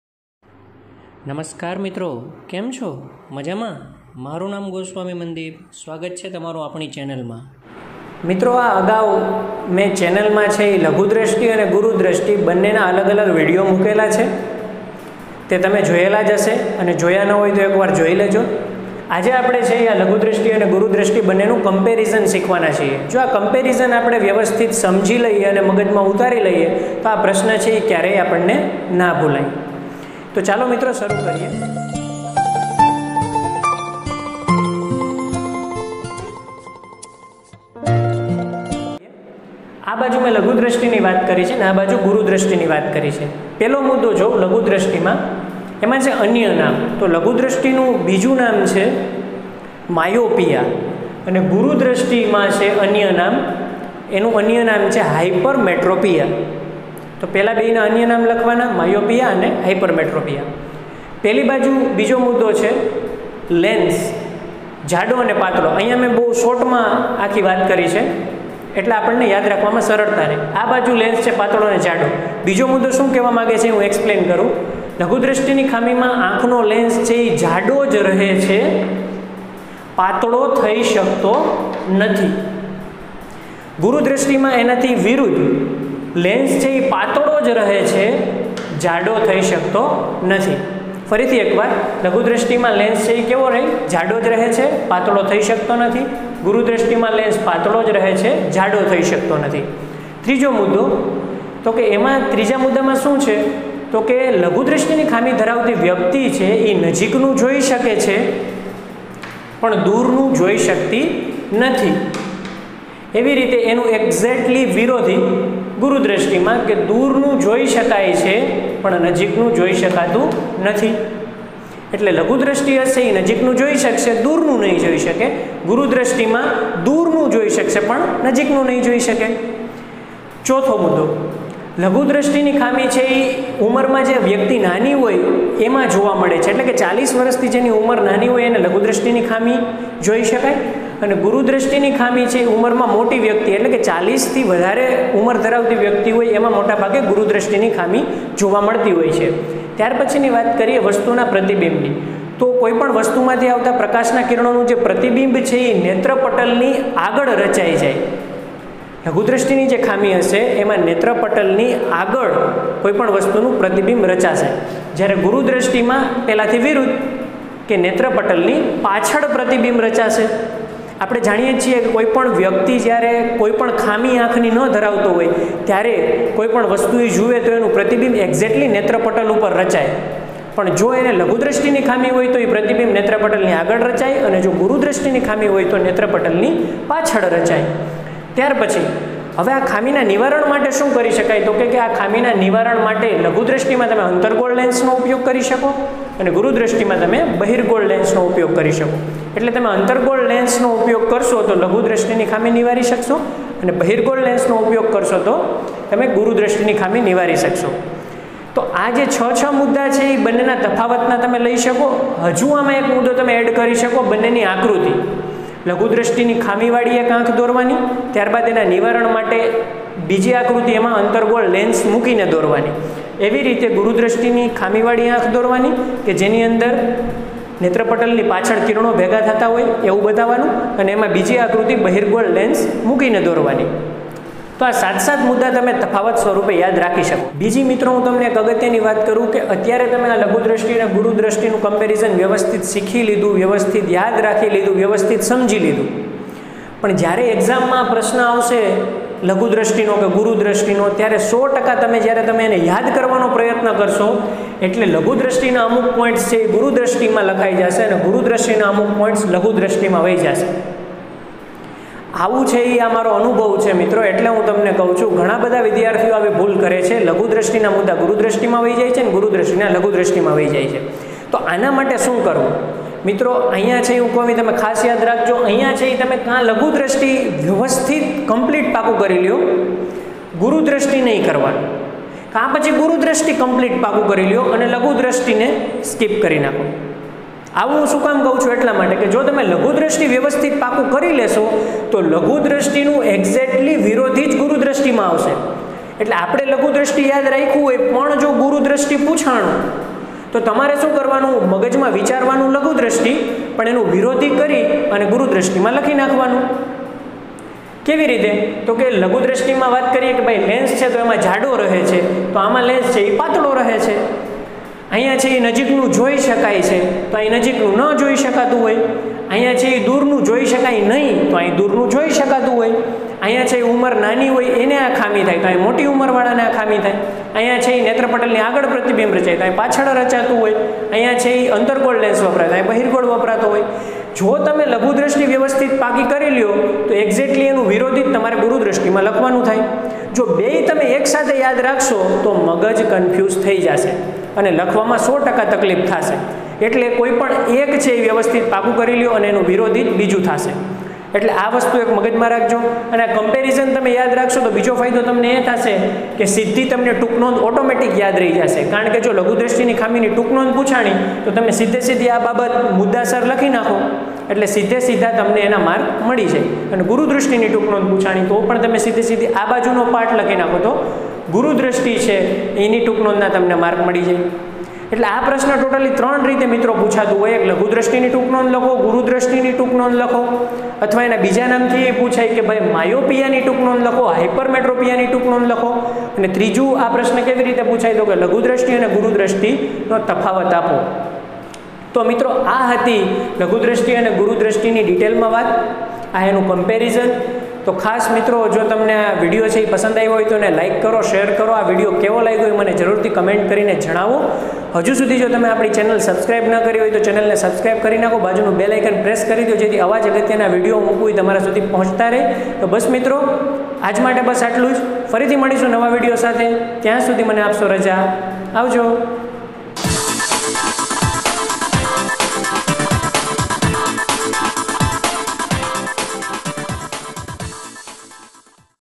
Comparison of Myopia and Hypermetropia|લઘુદ્રષ્ટિનીખામી અને ગુરુદ્રષ્ટિનીખામી નો તફાવત|happy to help
લઘુદ્રષ્ટિ અને ગુરુદ્રષ્ટિ બન્ને પ્રશ્નો ના જવાબ જ્યારે એકસાથે યાદ રાખવાના આવે ત્યારે વિદ્યાર્થીઓ હમેશા ભૂલ કરતા હોય છે અને બન્ને ખામીઓની માહિતી આડાઆવડી કરી નાખતા હોય છે ત્યારે આ વીડિયો એકદમ સરળ રીતે બન્ને ખામીઓ ને એકસાથે કાઈ રીતે લાંબાગાળા સુધી યાડ રાખી શકાય તે અંગે નું સચોટ માર્ગદર્શન આપવાનો પ્રયત્ન કરેલ છે...
જો તમને આ વીડિયો પસંદ આવે તો જરૂરીયાત ધરાવતા વિદ્યાર્થીઓ સુધી આ વીડિયોને અચૂક પણે SHARE કરજો.

                           -  Created By: Mandeepsir